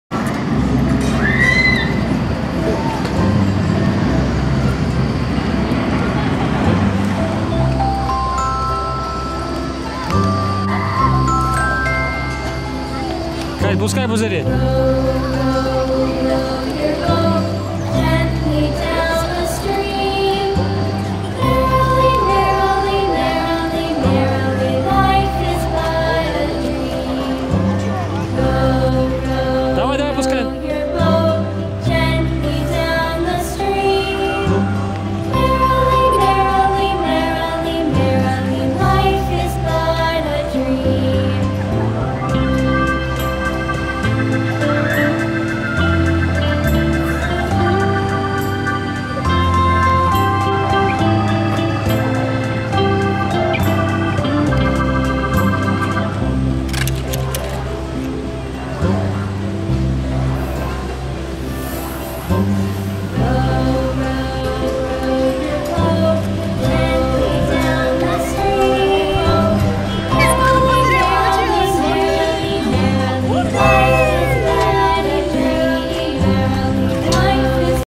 Мыльные пузыри для детей. Катя в парке аттракционов.
Катя снимает видео для детей. Надуваем мыльные пузыри в парке.  Как можно повеселиться?
Мыльные пузыри? Мыльные пузыри – интересное времяпрепровождения для всех. Летит мыльный пузырь! Ловите!